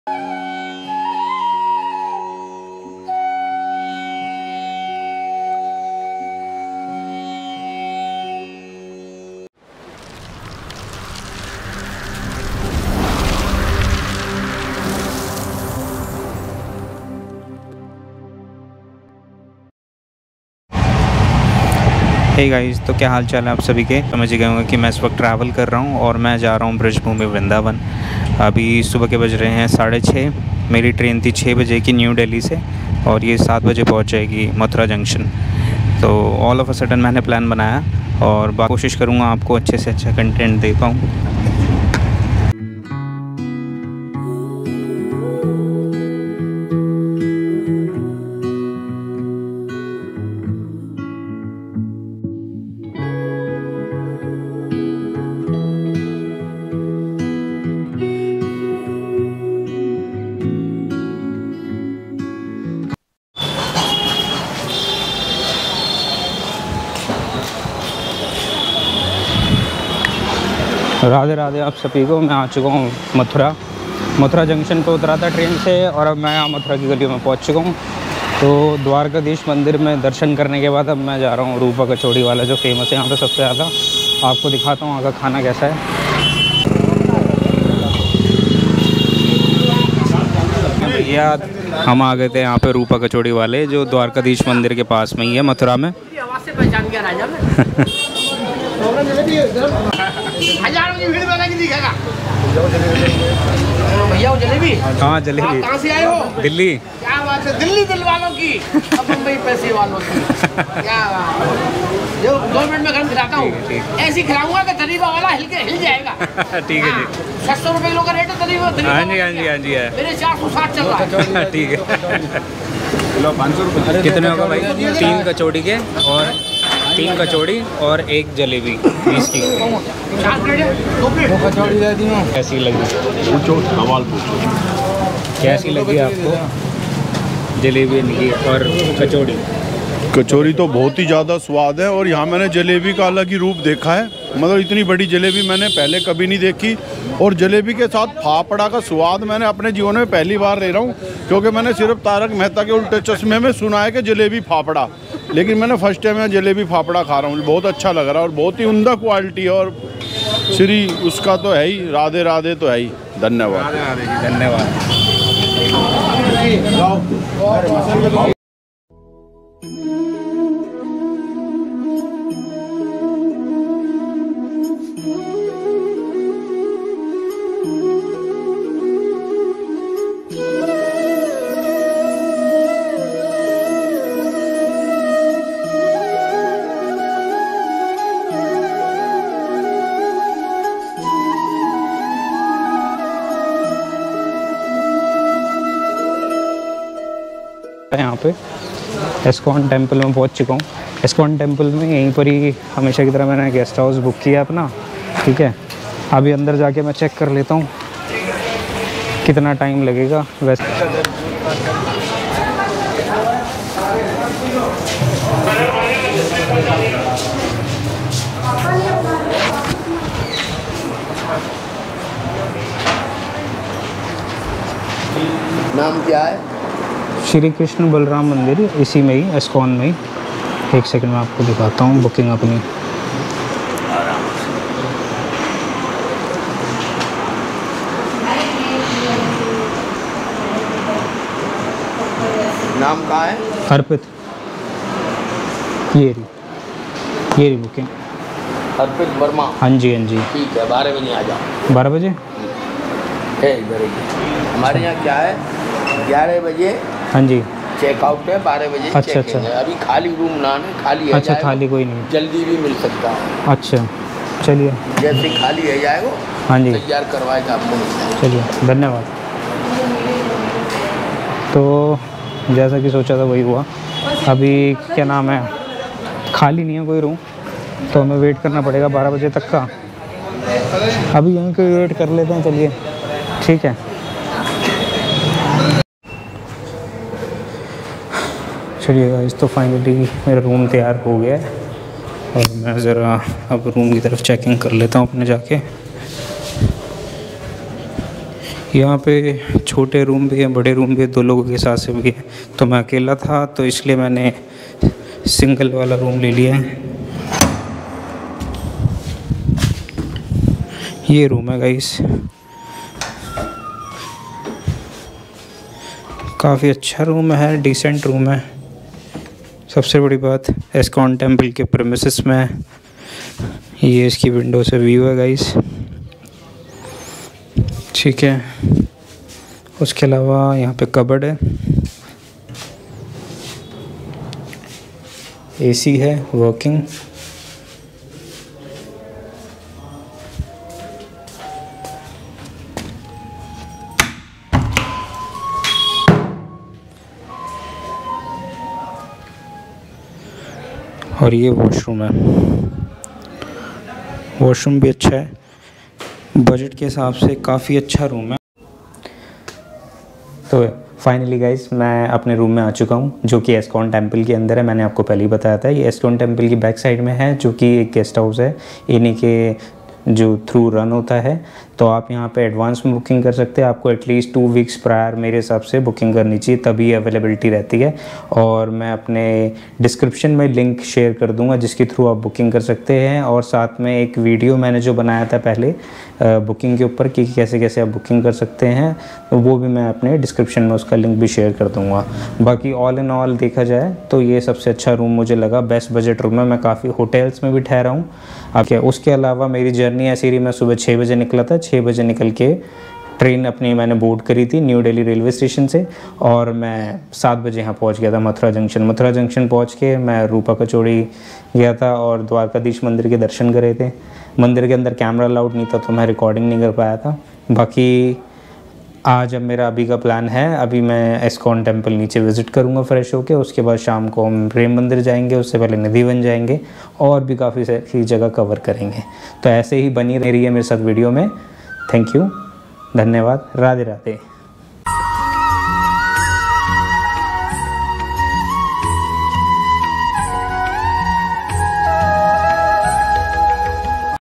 Delhi se Vrindavan| Iskcon guest House| Rupa kachori wale
You can use the above link to book accommodation in Iskcon temple Vrindavan.
Please make sure to book at least two weeks prior.